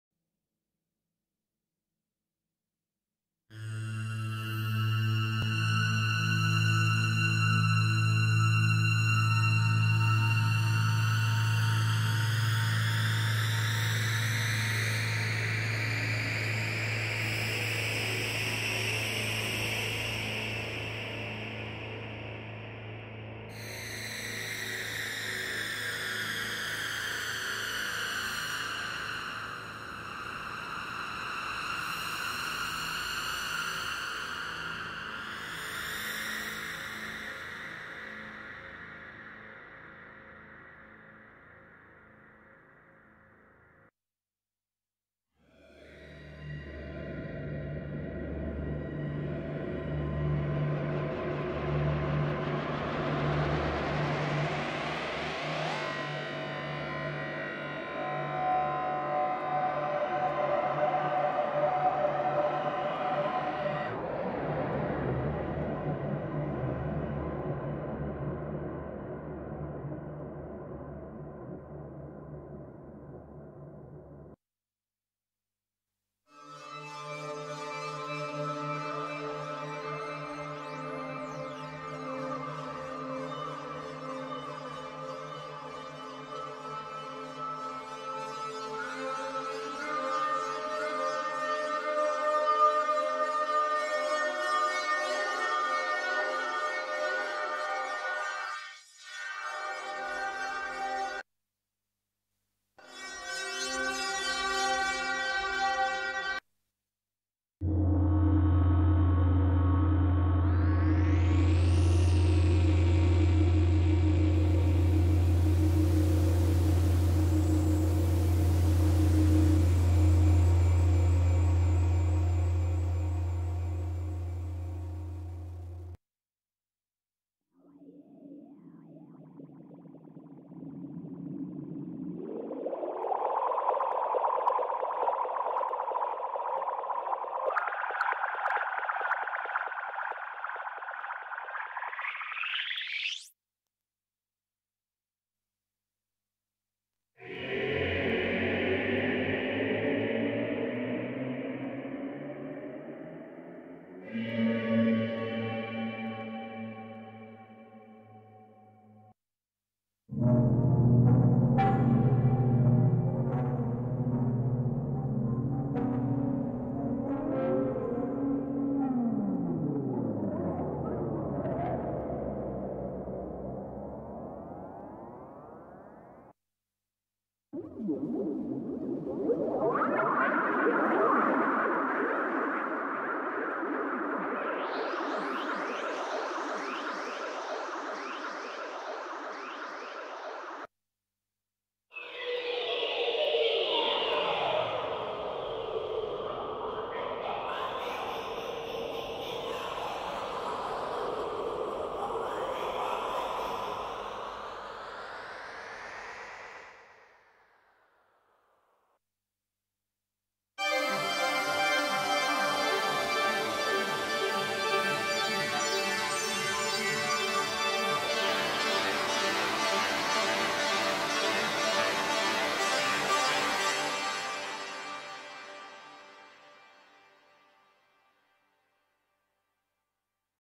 Rapid Synth v1.8 VST | Atmospheric presets (no talking)
Parawave Rapid Synth v1.8 VST | Atmospheric presets | Realtime routing modulation